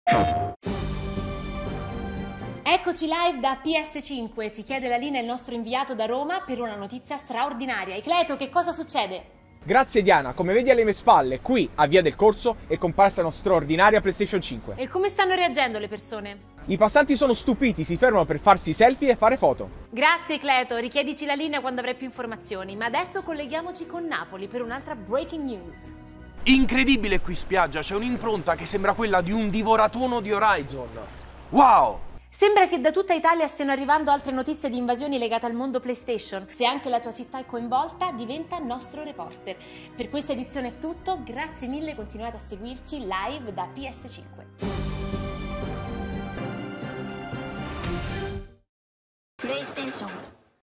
Ultime notizie dall'Italia | Live From PS5
Eccoci LiveFromPS5 con Diana Del Bufalo: una console PS5 è apparsa a Via del Corso a Roma. Dai nostri inviati @ilsolitomute e @ecletoh continuano ad arrivare notizie di avvistamenti di titoli PlayStation nel nostro mondo. Diventa anche tu un reporter